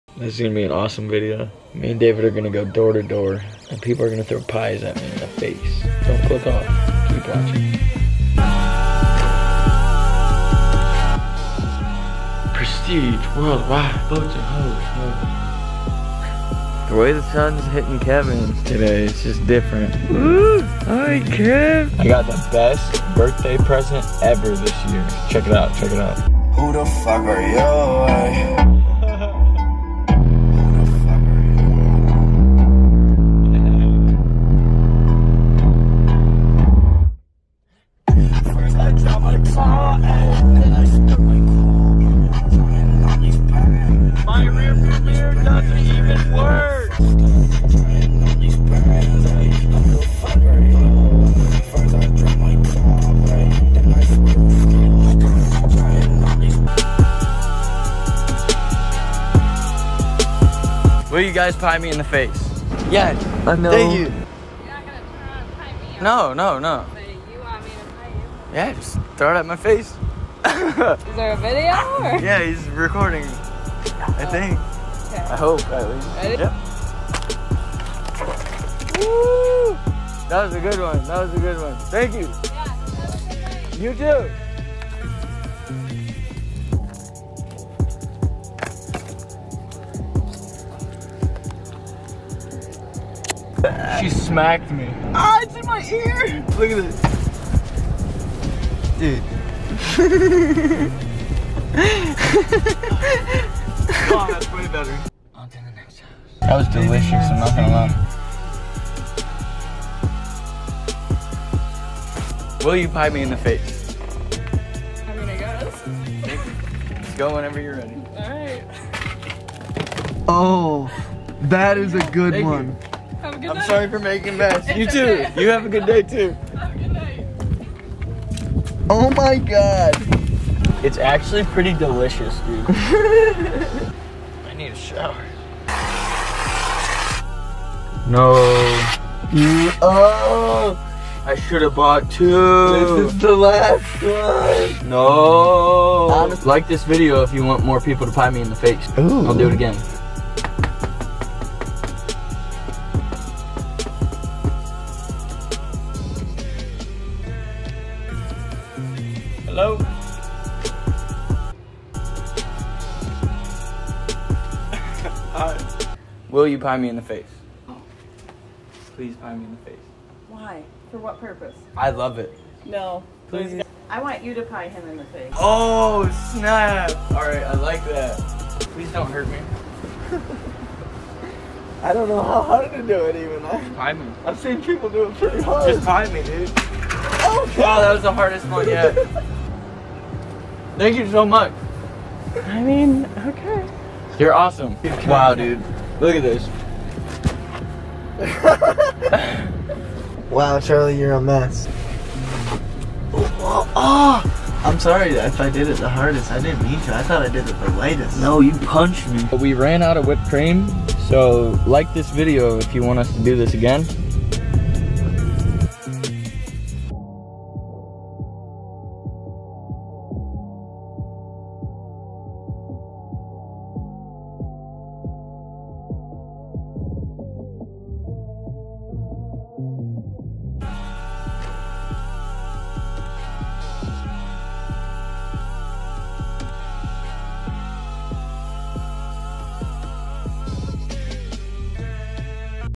Strangers cream pie me in the face!
Ft. Three kind strangers that smacked me in the face with some cream pies and David.
I post every Tuesday, Friday and Sunday
I ran out of whipped cream so if you want me to do this again drop a like or a comment and I will happily do this again!